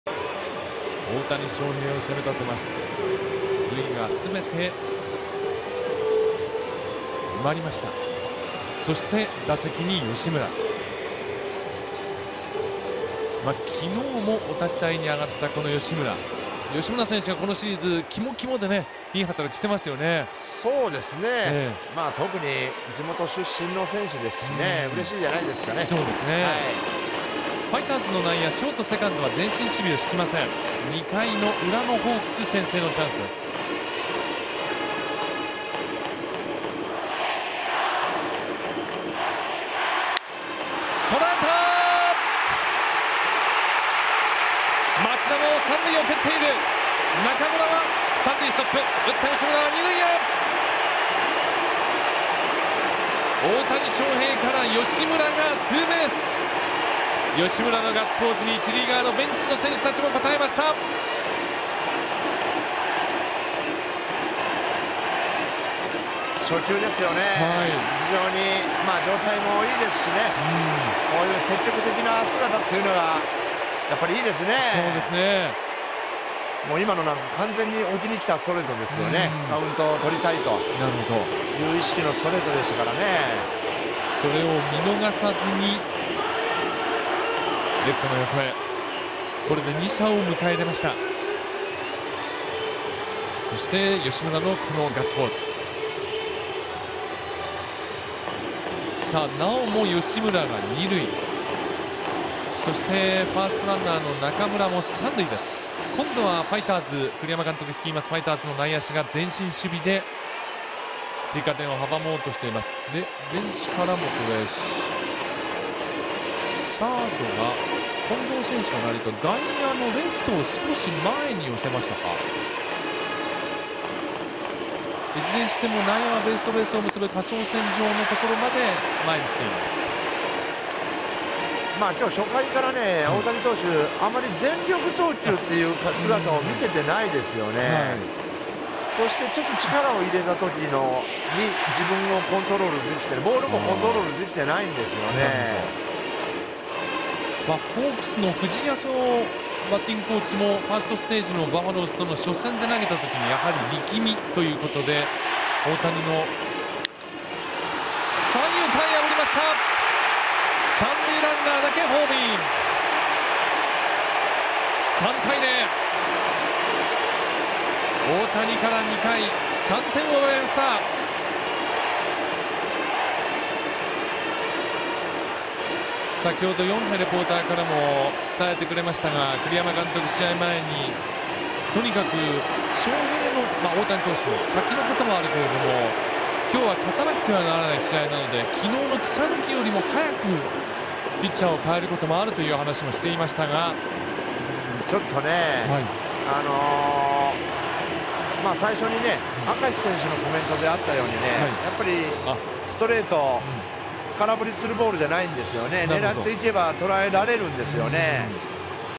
【プロ野球パ】大谷から先制点！吉村、今宮の連続タイムリーで3点奪取 2014/10/19 H-F
2回、無死満塁の大チャンスに、このシリーズでは大事な場面での活躍が続く吉村に打順がまわる。ここも吉村は期待に違わぬタイムリーツーベースで2点を先制。さらに今宮も続いてタイムリーを放ち、ソフトバンクが大谷をまずは捉えた。（10月19日 ソフトバンク-日本ハム）